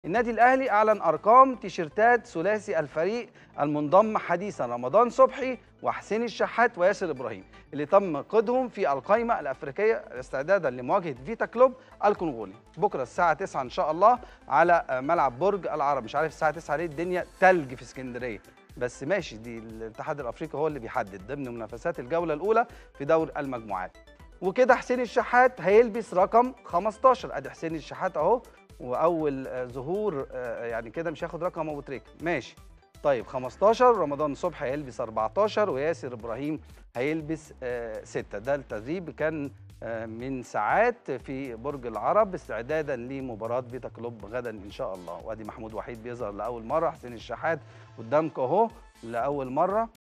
الأهلي يُعلن أرقام قمصان رمضان صبحي وحسين الشحات وياسر إبراهيم بعد قيدهم إفريقياً
اللعيب | الأهلي يُعلن أرقام قمصان رمضان صبحي وحسين الشحات وياسر إبراهيم بعد قيدهم إفريقياً